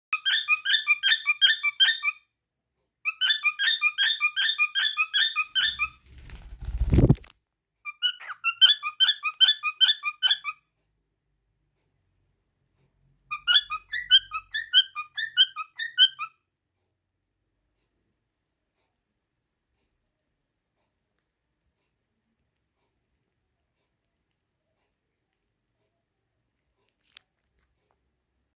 A cute little parrot is singing - Funny Birds
This little cockatiel likes singing. The bird's voice is amazing. The parrot sings well. Just look at this little bird and enjoy the video.